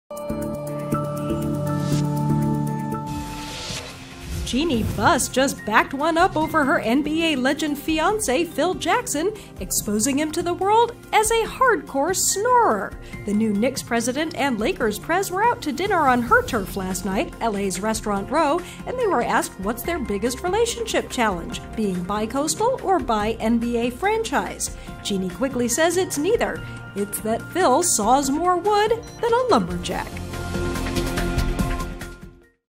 Phil Jackson & Jeanie Buss - He's So Loud In Bed
Jeanie Buss just backed one up over her NBA legend fiance Phil Jackson -- exposing him to the world ... as a hardcore snorer! The new Knicks President and Lakers Prez were out to dinner on her turf last night -- L.A.'s restaurant row -- and they were asked what's their biggest relationship challenge: being bi-coastal or bi-NBA franchise?
Jeanie quickly says it's neither ... it's that Phil saws more wood than lumberjack.